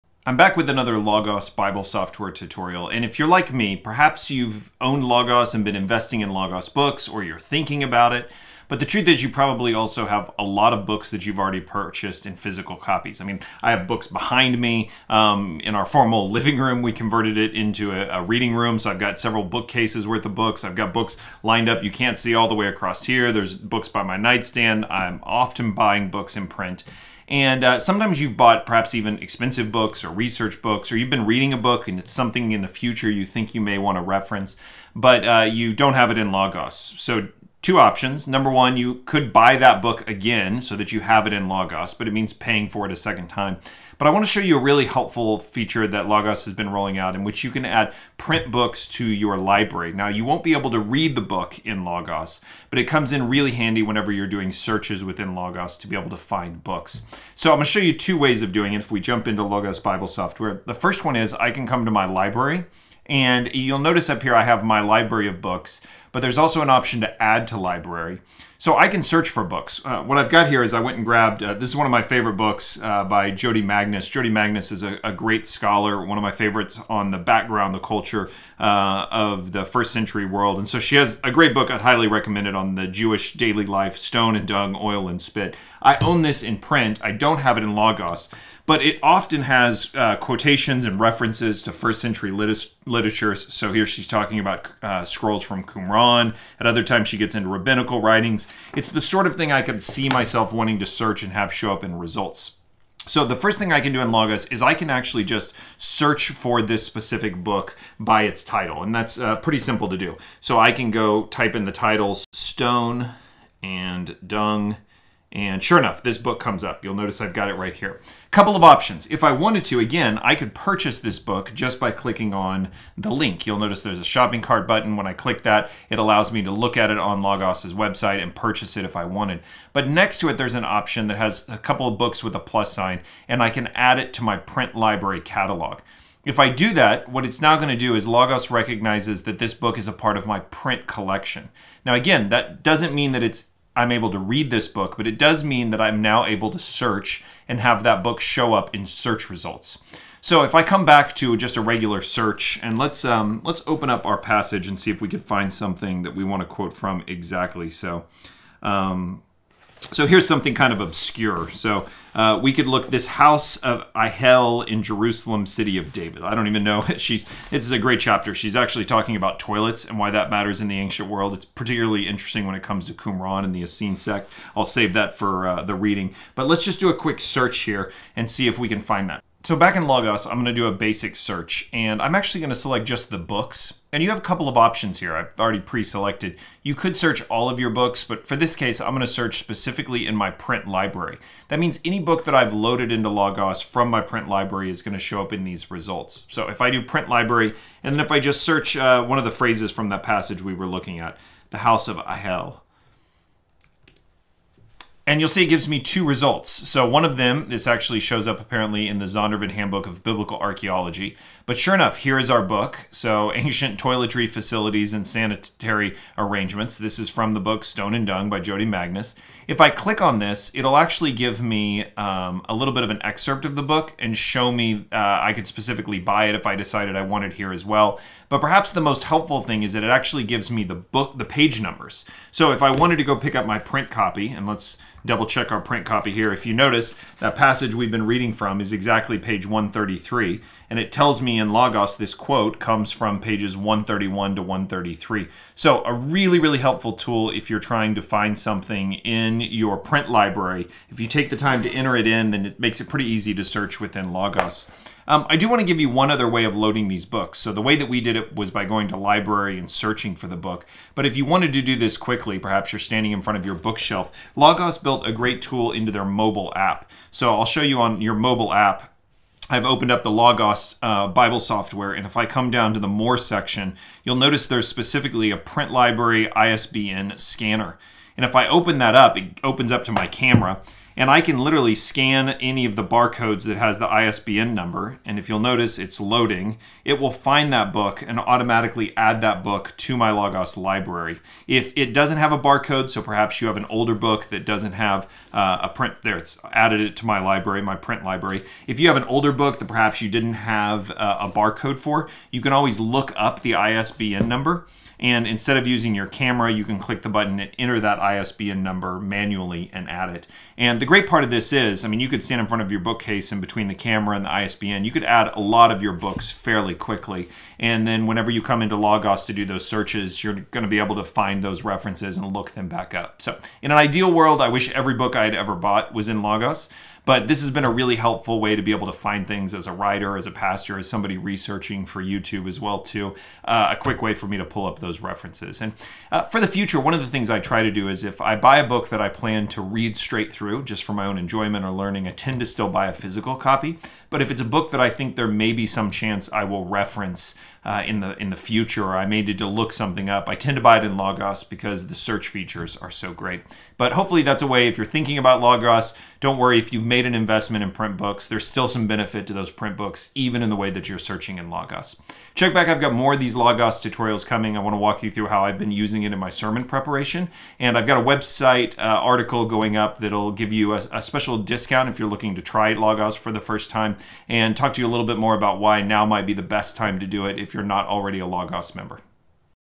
How to add your print books in Logos Bible Software
Over the years, I've bought a lot of books. They are scattered across our house. But I've also bought a lot in Logos. The key to Logos is building a great library. Logos Bible Software is a great tool, but it's only as good as the library to which it has access.

Logos has recently rolled out a tool to help integrate the print books you already own with the powerful search tools of Logos Bible Software. In this tutorial, I'll show you how you can take advantage of it.

See how you can get a free 30-day trial of Logos.